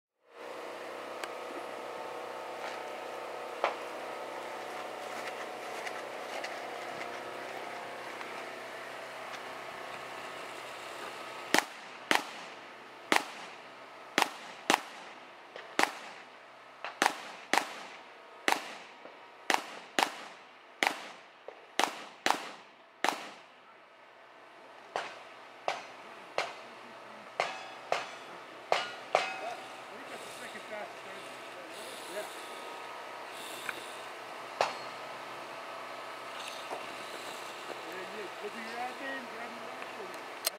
2019 AZO MIND & BODY RUN & GUN || STAGE 2 || ADAM LOWE || T Smith Armory
2019 AZO Mind and Body Run and Gun
Stage 2
Shooter: Adam Lowe